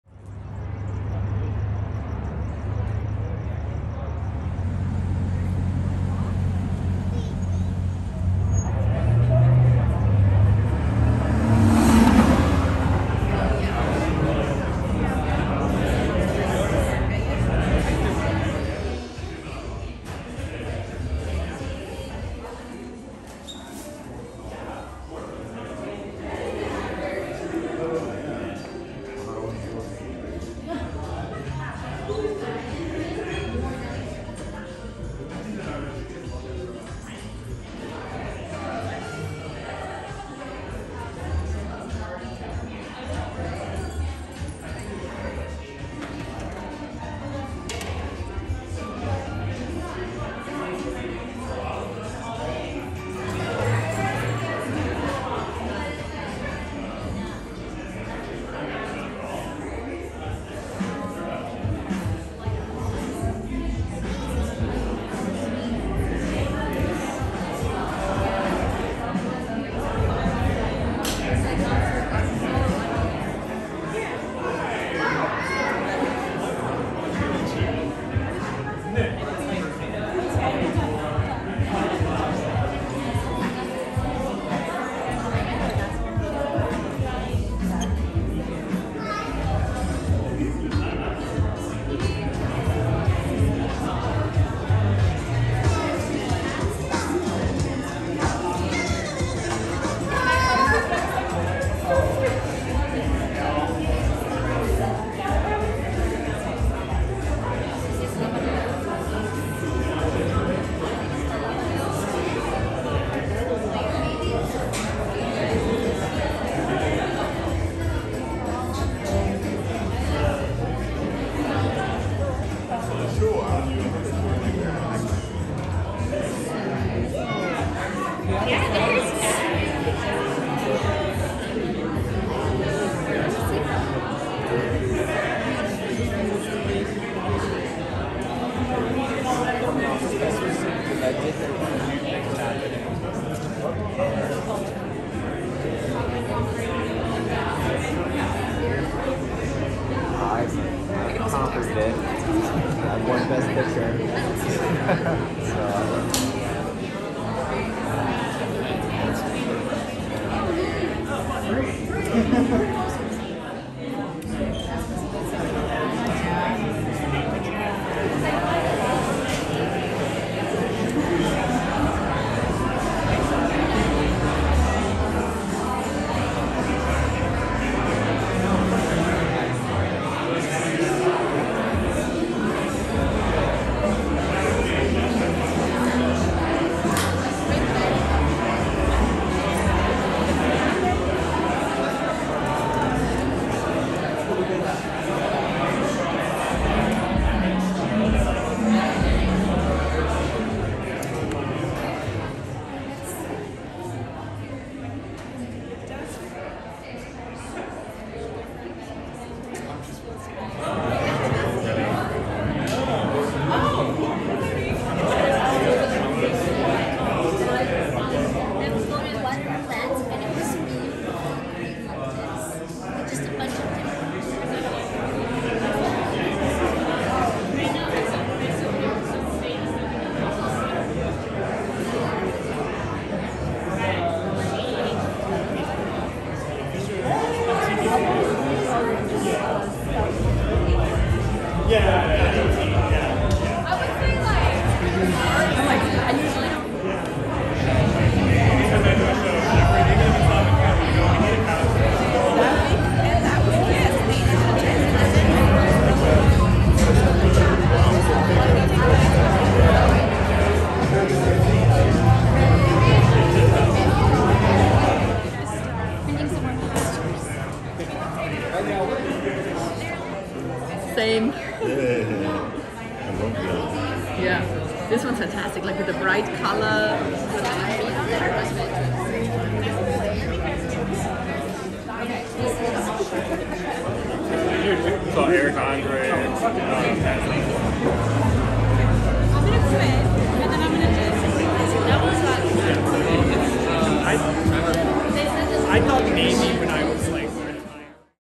Nicholas Bono Kennedy: Sunset Nursery / Simchowitz Pasadena
Nicholas Bono Kennedy's exhibition "Sunset Nursery" at Simchowitz Pasadena from February 23rd to March 29th, 2024, delves into humanity's impact on the environment. Through paintings exploring the balance between creation and destruction, Kennedy prompts reflection on human intervention in nature. Depicting interior plant life, he highlights our detachment from the natural world despite its incorporation into our homes. Inspired by artists like Ken Price, Norman Blume, and Georgia O’Keeffe, Kennedy pushes artistic boundaries, emphasizing light's role in shaping spatial perceptions and emotional experiences. The exhibition urges viewers to contemplate the unintended ecological consequences of human actions and the tendency towards narcissism in our endeavors.

Nicholas Bono Kennedy: Sunset Nursery. Solo exhibition at Simchowitz Pasadena. Pasadena, February 23, 2024.